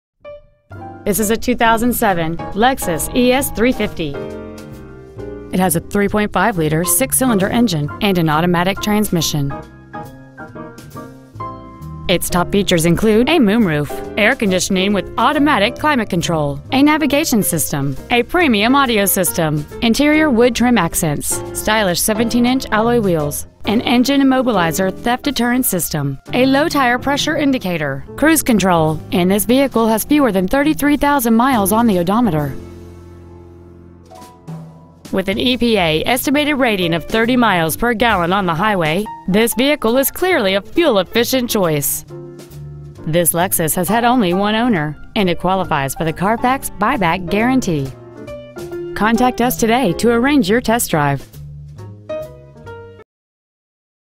2007 Lexus ES 350 Carrollton TX 75006
Year : 2007

 Make : Lexus

 Model : ES 350

 Engine : 3.5L SFI 24-valve V6 engine

 Trans . : Automatic

 Exterior : White

 Miles : 32,199

 Interior : Tan

 eCarOne

 1875 N. Interstate 35E

 Preowned 2007 Lexus ES 350 Carrollton TX